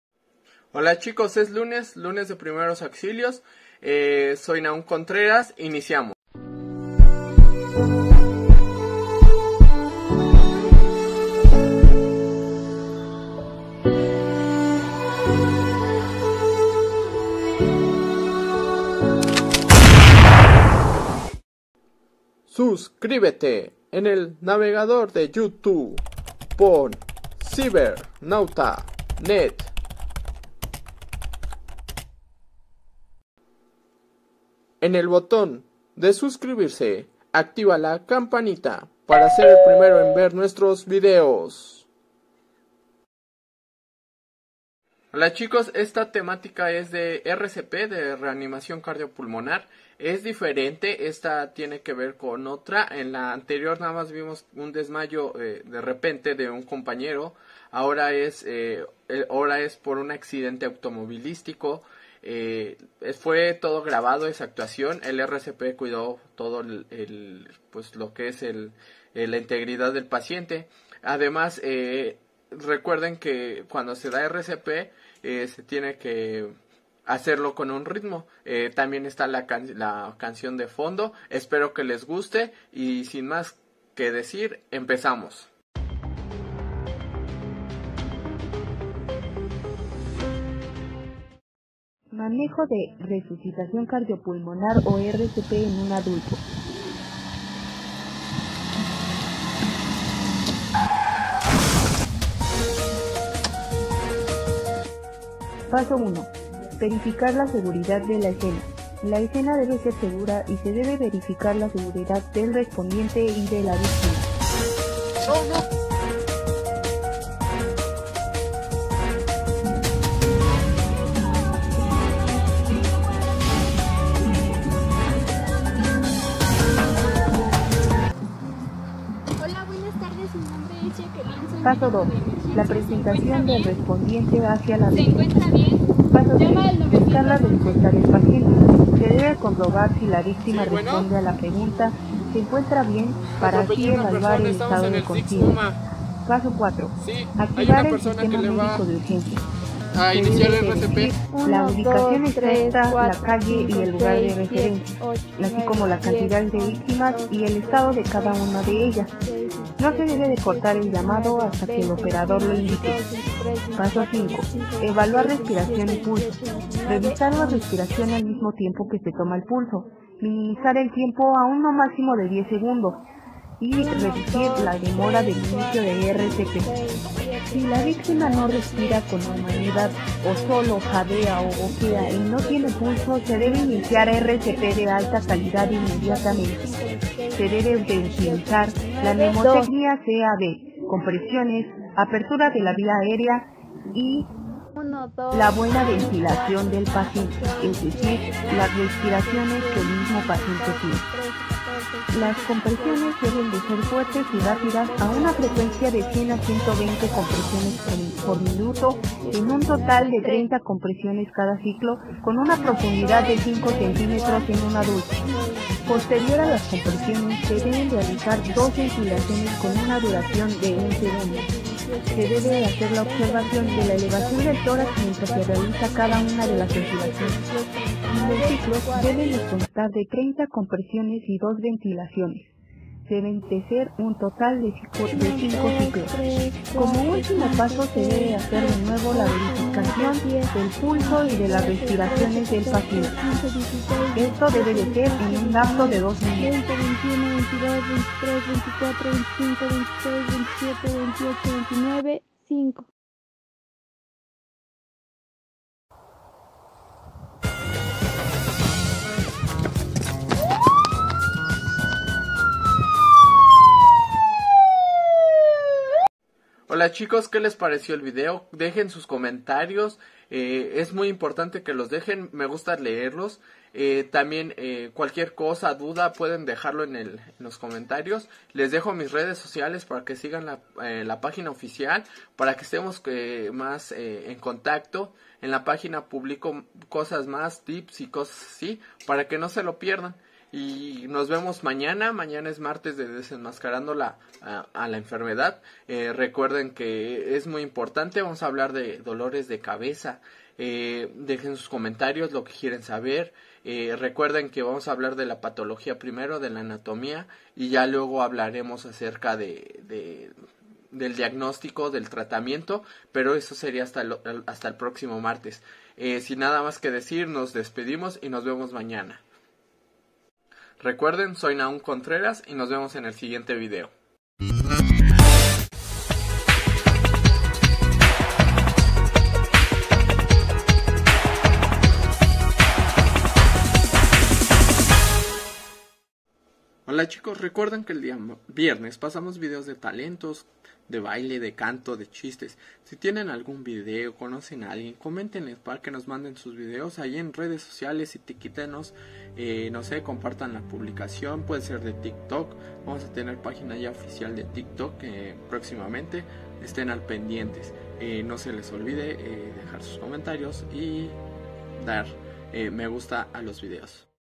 Primeros Auxilios Cap 3
En este capitulo de primero auxiolos aprenderas como dar RCP, ya veriguaras por que Nahum es buscado por la justicia.
Las preguntas es ¿pagara a la justicia?
no te pierdas el vídeo y deja tus comentarios.

REDES OFICIALES
Facebook CIBER NAUTANET